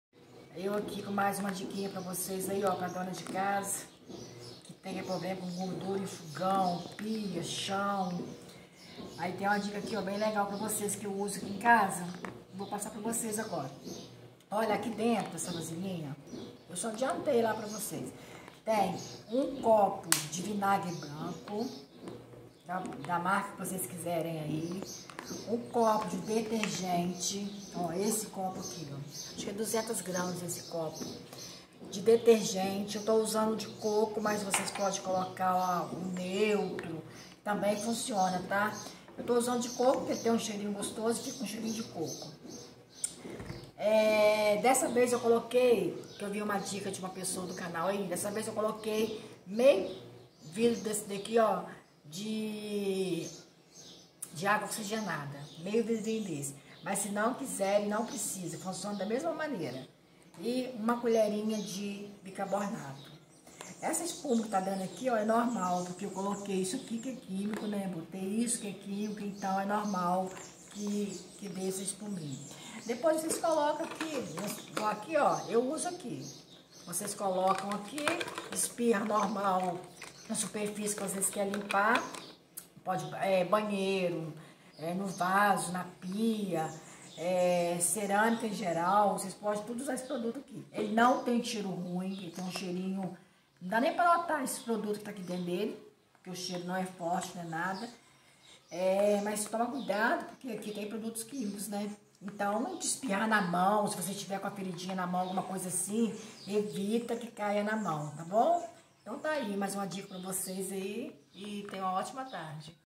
Receita caseira de produto de limpeza doméstica.
Produto feito em casa com quatro ingredientes super barato e eficiente na limpeza doméstica.